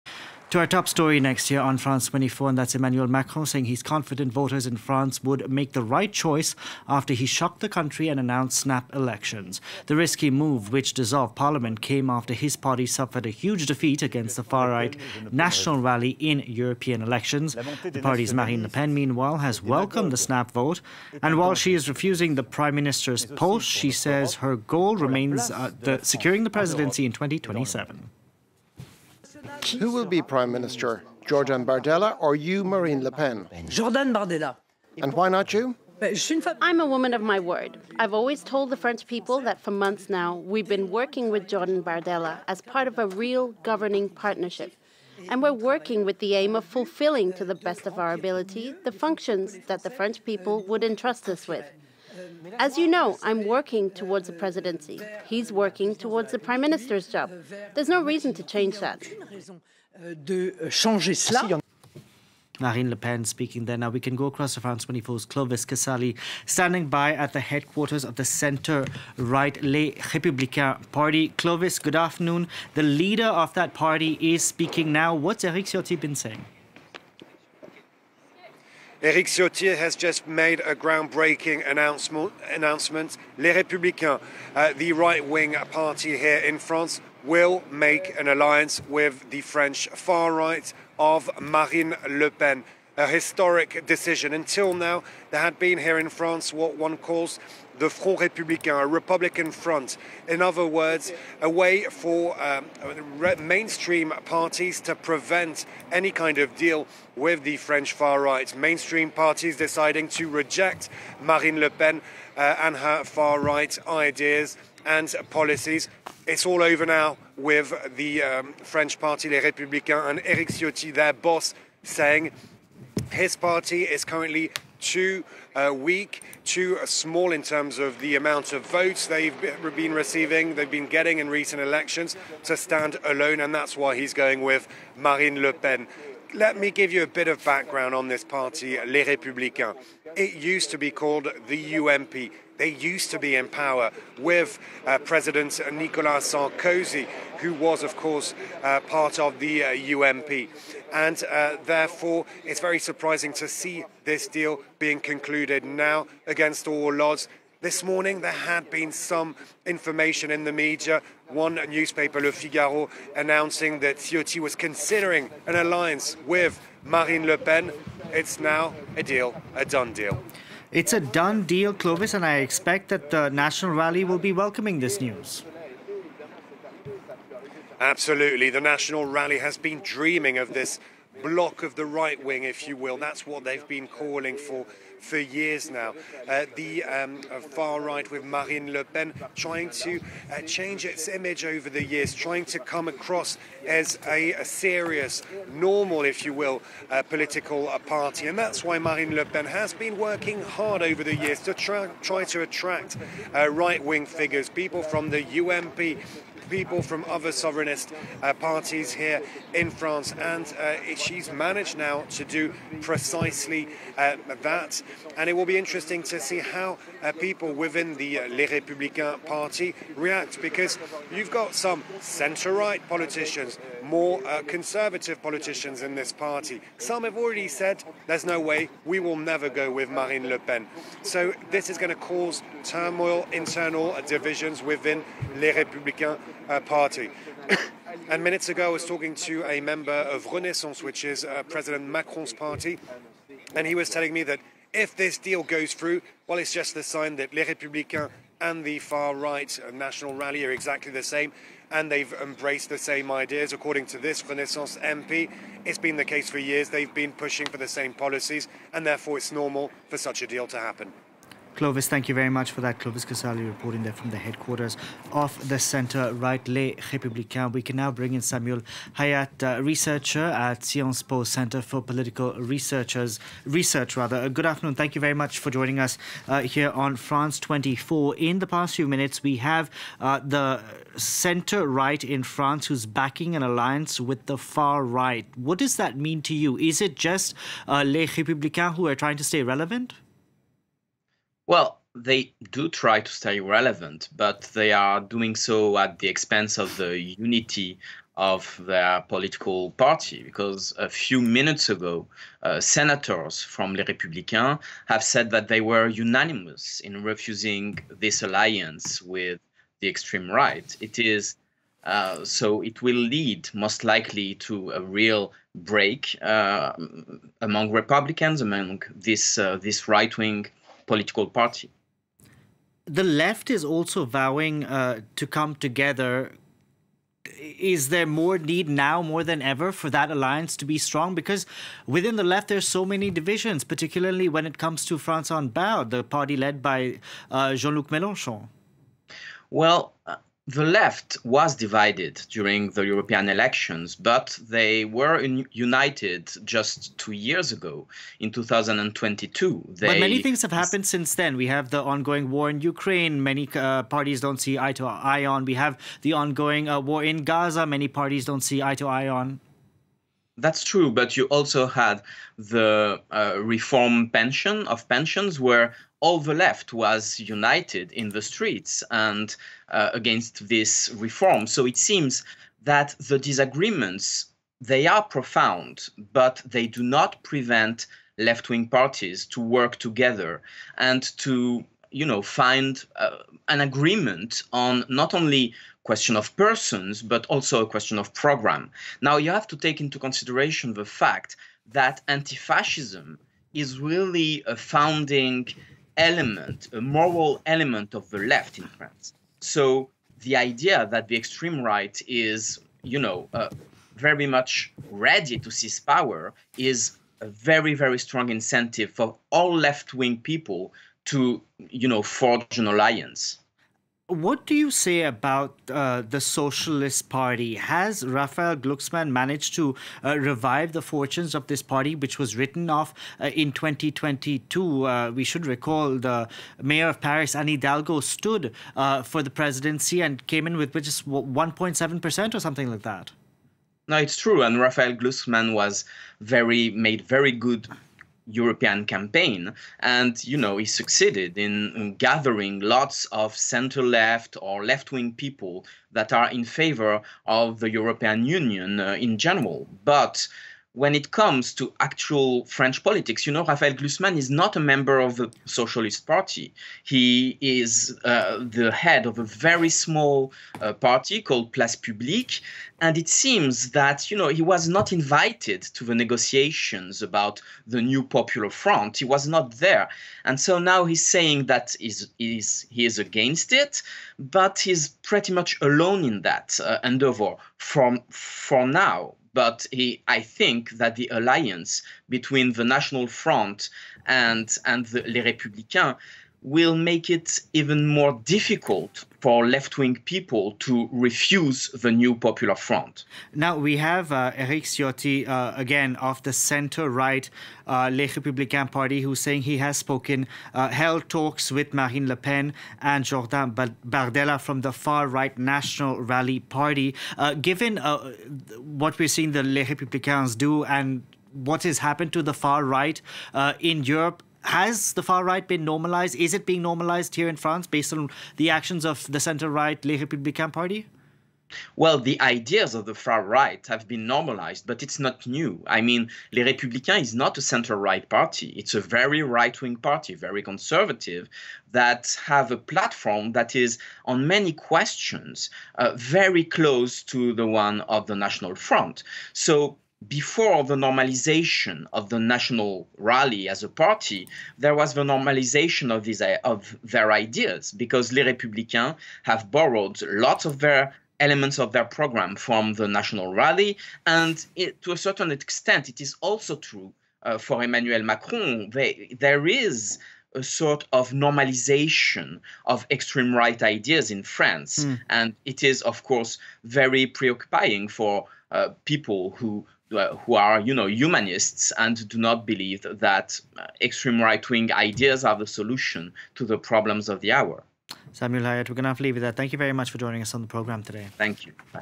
Les Républicains: 'Very right-wing party' whose platform is 'very close to that of the far-right RN'
The leader of France's traditional right-wing party on Tuesday backed an alliance with the far right of Marine Le Pen in snap legislative elections, triggering a crisis within his own party and fury from the government. The stunning announcement by the Republicans (LR) leader Eric Ciotti in a television interview is the first time in modern French political history that a leader of a traditional party has backed an alliance with the far-right National Rally (RN). For in-depth analysis and a deeper perspective on the French conservative party leader's highly controversial move to establish an alliance with the far right for the snap polls, FRANCE 24's Delano D'Souza is joined by fellow reporter Clovis Casali and guest analyst Samuel Hayat, French Political Scientist and CNRS Research Fellow at CEVIPOF-Sciences Po.